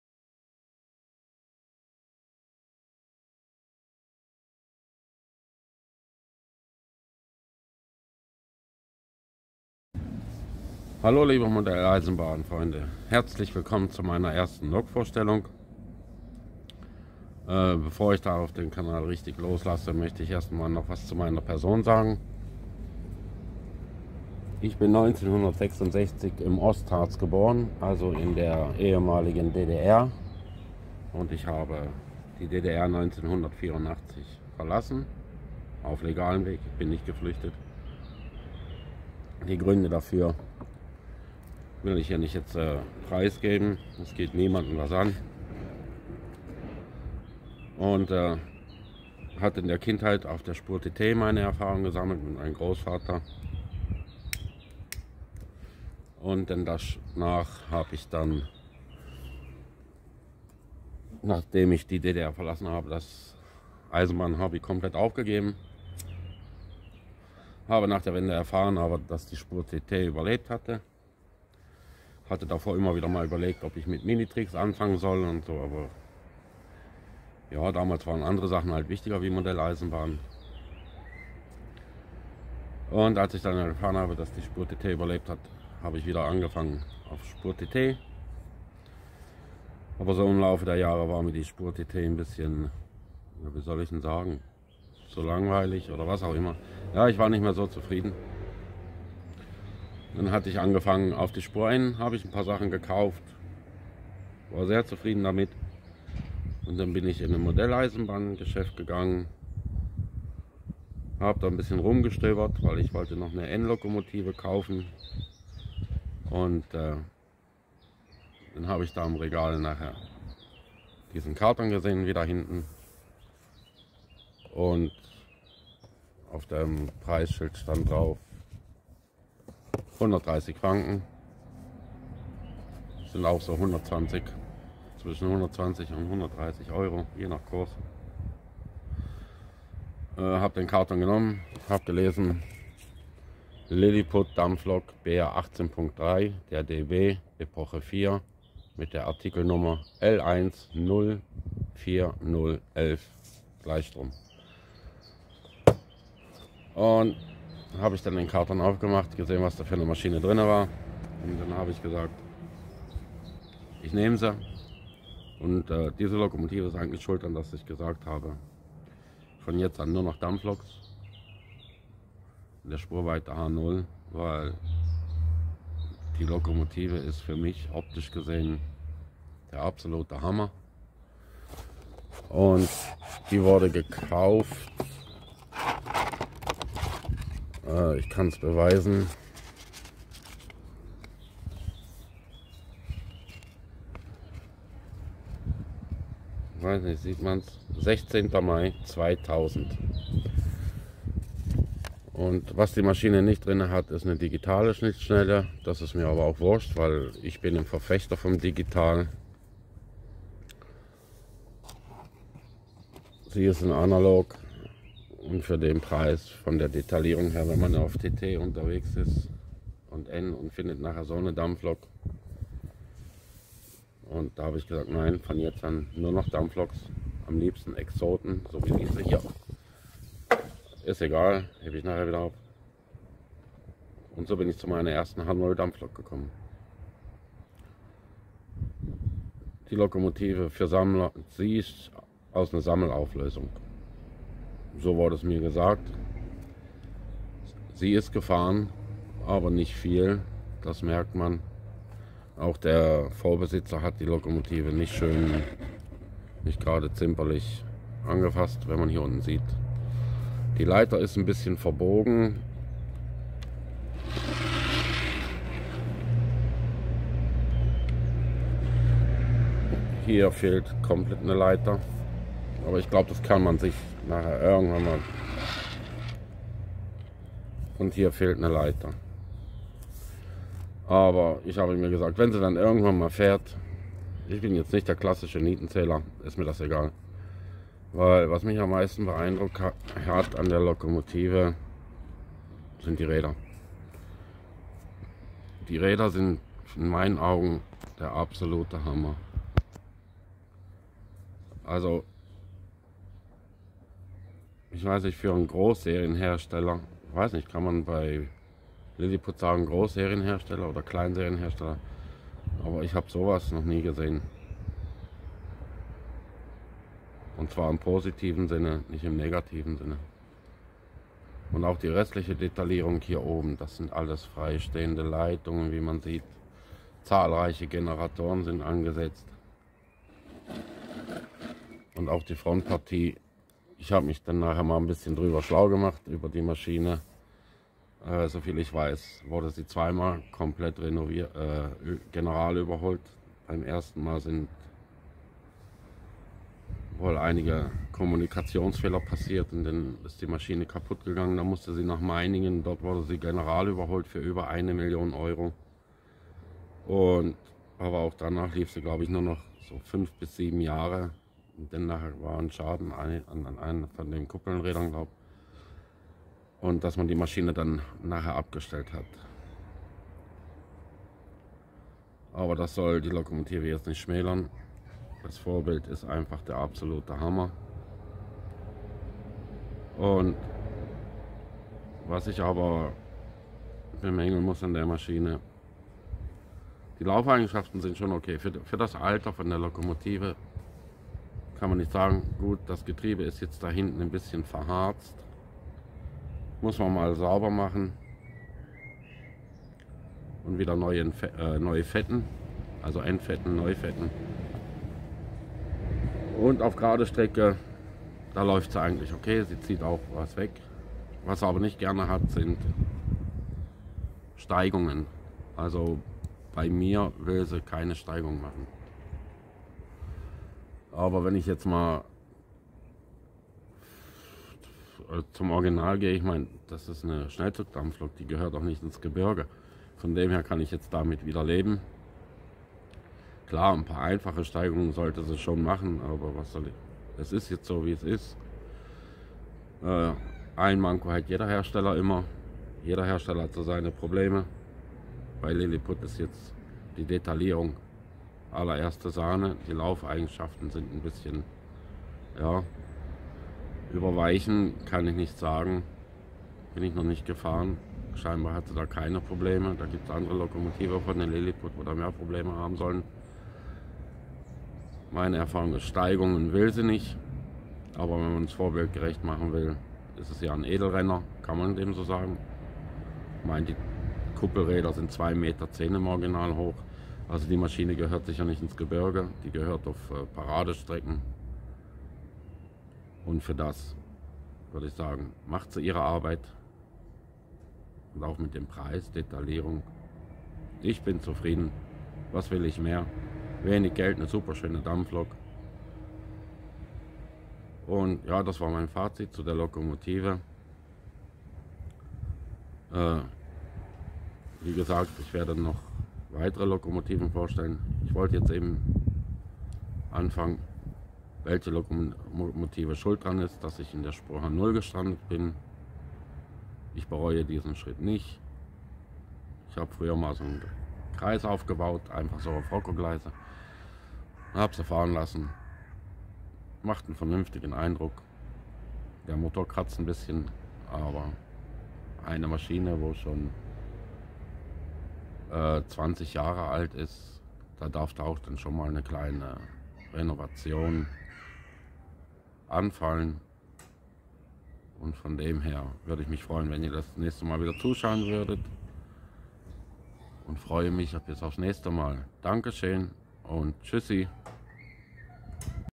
Gebrauchte Modelleisenbahn H0 (1). Liliput BR 18.3 DB
Hallo Modellbahner.
In diesem Video stelle ich euch die Dampflok BR 18.3 in H0 von Liliput in der Spurrweite H0 vor. Gekauft wurde sie bei einem Modellbahn Händler per Zufall günstig bekommen habe.